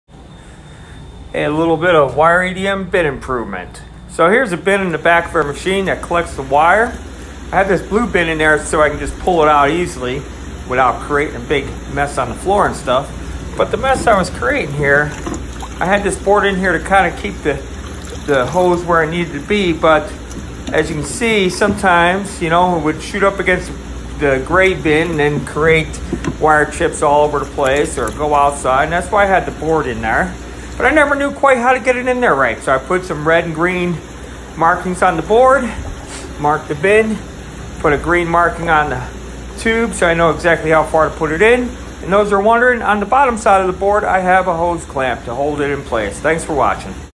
2 Second Lean, EDM bin Improvement
Added a board with a hose clamp, that is marked to help get the house in the right position every time.